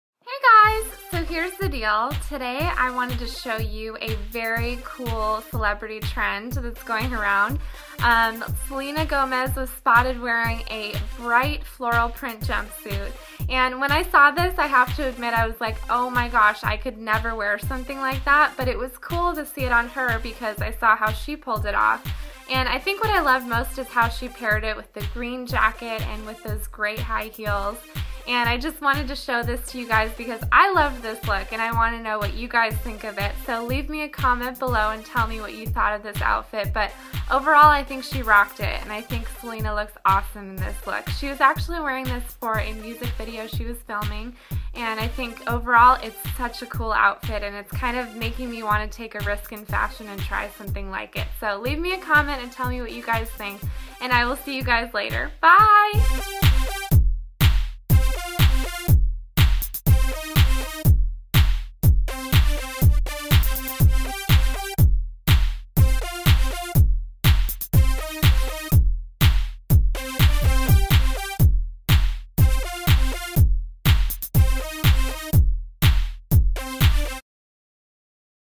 Celebrity Trend Alert- See What Selena Gomez is Wearing!
Sign up for monthly fashion tips from me!

Hey loves!
Selena wore a crazy bright jumpsuit the other day, and I just had to show you this outfit of hers! Turns out, another celebrity was spotted wearing the same exact jumpsuit! Which one do you think pulled it off?

XOXO
Lots of Love,
Eryka Couture